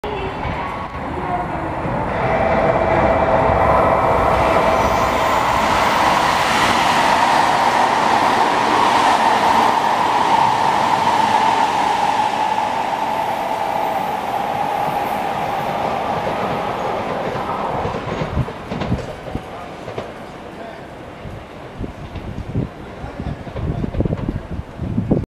特急『ソニック28号』博多行(3028M) 883系[AO-1](7両編成) 吉塚駅通過
★撮影場所: JR吉塚駅
★列車名:『ソニック 28号』
★列車番号: 3028M
★編成:7両編成[AO-1](883系)『青いソニック』
★停車駅(日豊本線･鹿児島本線経由)
大分―Oita
別府―Beppu
亀川―Kamegawa
杵築―Kitsuki
宇佐―Usa
柳ケ浦―Yanagigaura
中津―Nakatsu
宇島―Unoshima
行橋―Yukuhashi
小倉―Kokura
戸畑―Tobata
黒崎―Kurosaki
折尾―Orio
香椎―Kashii
博多―Hakata